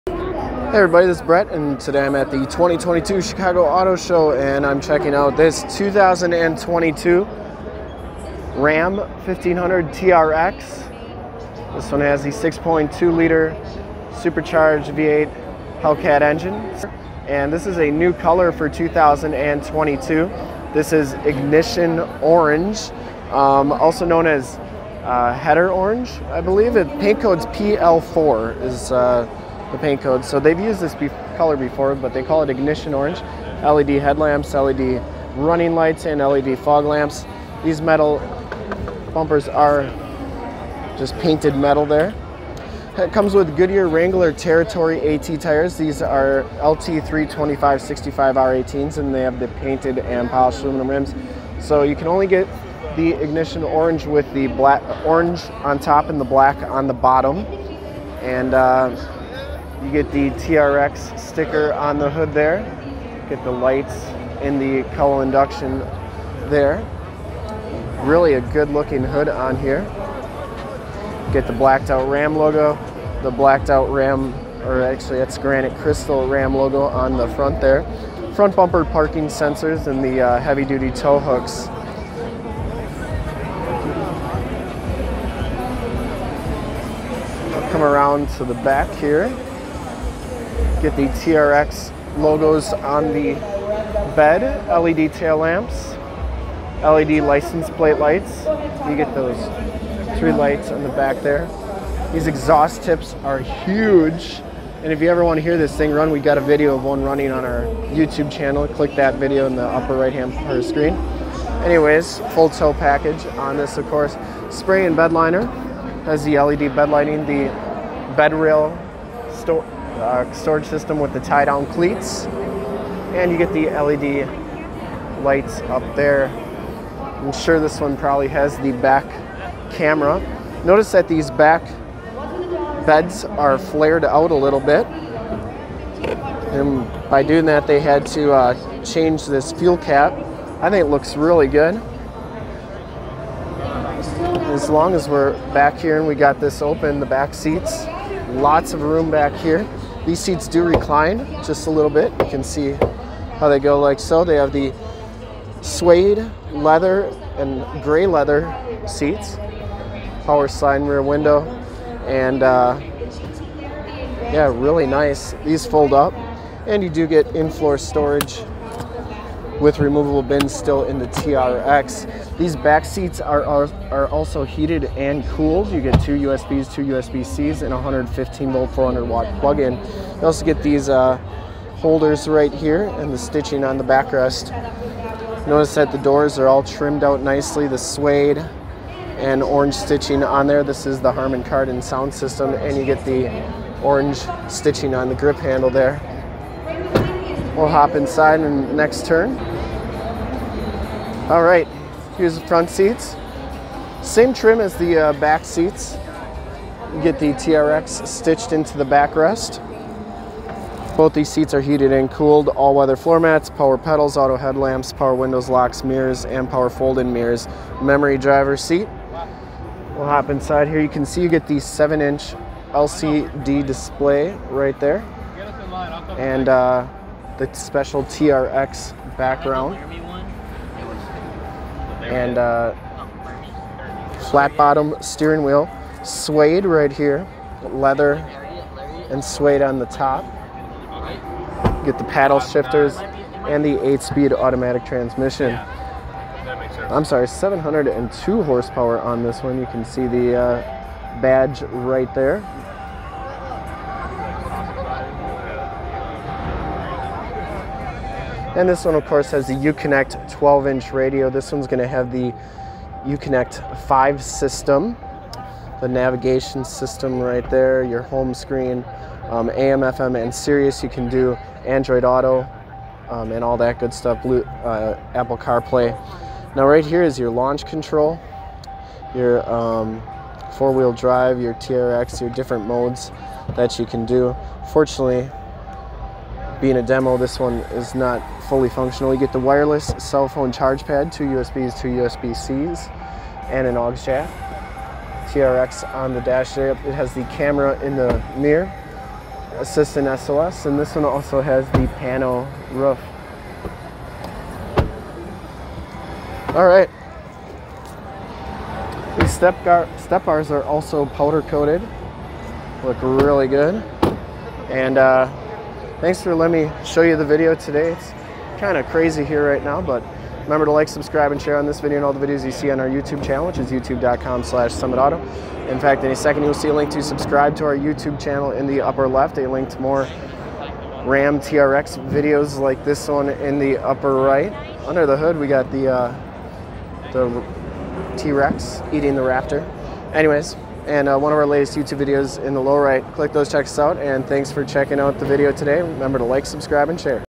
2022 RAM 1500 TRX IGNITION ORANGE WALKAROUND OVERVIEW FIRST LOOK 2022 CHICAGO AUTO SHOW
THIS 2022 RAM 1500 TRX IN IGNITION ORANGE WAS THE TRUCK I DID A WALKAROUND OVERVIEW OF AT THE 2022 CHICAGO AUTO SHOW

STOCK:
PRICE: $
MILES:
MAKE:
MODEL:
VIN:
LOCATION: FOND DU LAC OSHKOSH WISCONSIN, 54937 TRUCKS ON 41